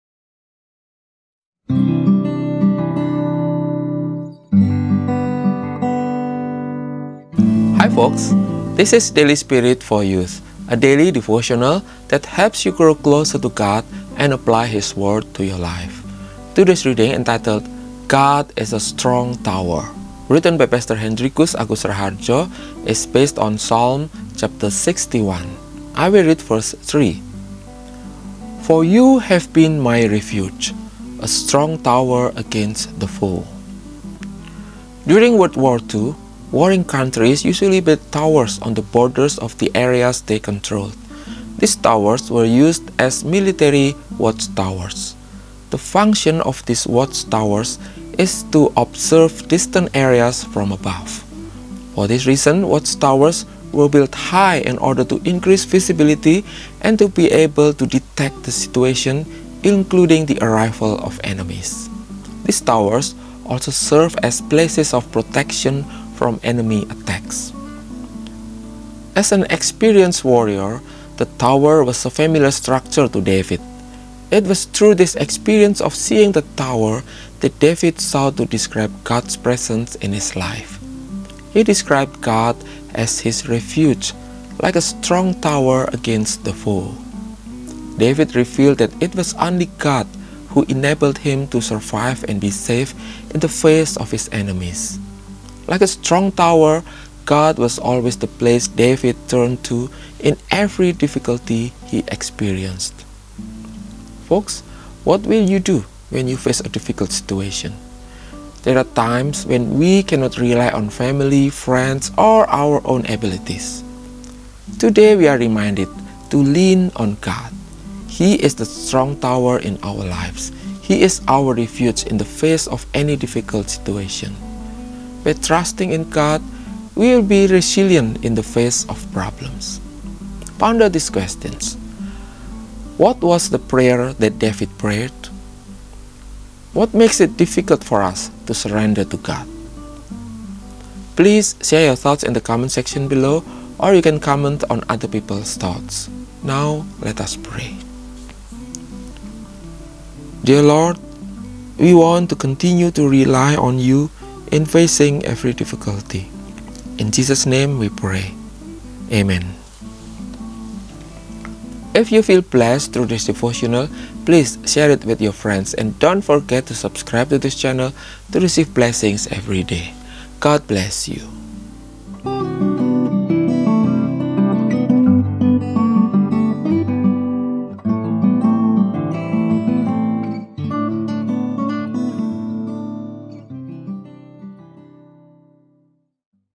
DAILY SPIRIT FOR YOUTH 281 - GOD IS A STRONG TOWER
Yayasan Komunikasi Bersama - GKI Present
Daily Devotional That Helps You Grow Closer To God And Apply His Word To Your Life
Based on Psalm 61
Script By: Pdt. Hendrikus Agus Rahardjo
Hosted by : Daniel Budilaksono